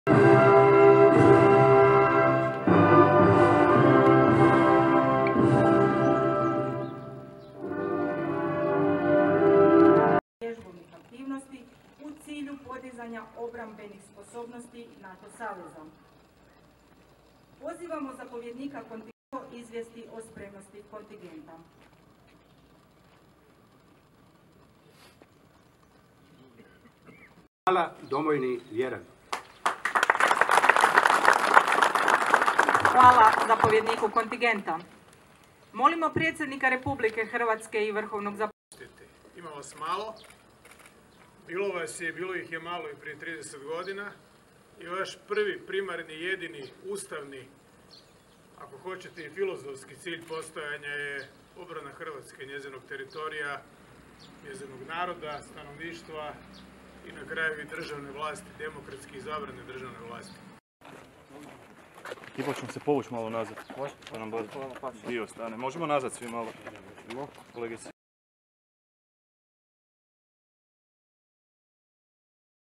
Predsjednik Milanović sudjelovao na svečanom ispraćaju 1. kontingenta OSRH u Mađarsku
Predsjednik Republike i vrhovni zapovjednik Oružanih snaga Republike Hrvatske Zoran Milanović sudjelovao je u vojarni 110. brigade Hrvatske vojske na svečanom ispraćaju 1. hrvatskog kontingenta u sklopu aktivnosti ojačane budnoćnosti NATO-a, u okviru borbene skupine pod vodstvom Mađarske, u Mađarsku